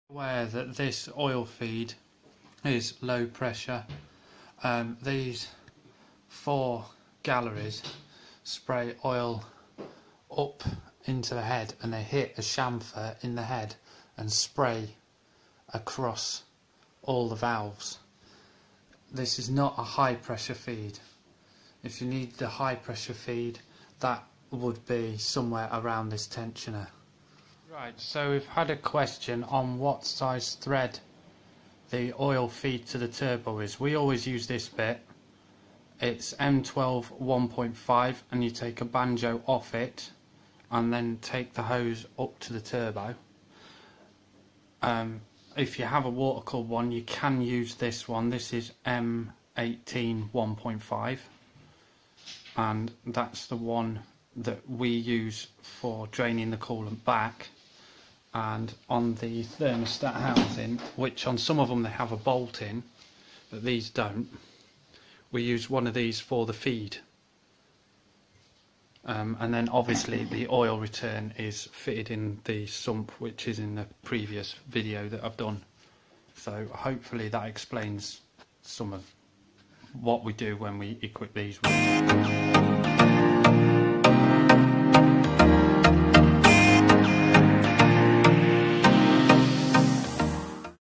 Mercedes M111 Turbo Oil Feed How To Guide... (size of fittings needed in description)
this video shows where to get the oil feed and coolant feed and return for fitting a turbo to the modern classic Mercedes Benz 230 kompressor m111 engine. how to fit a oil feed for a turbo for a Mercedes m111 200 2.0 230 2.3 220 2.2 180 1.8 including Kompressor/supercharged engines

oil feed thread size m12x1.5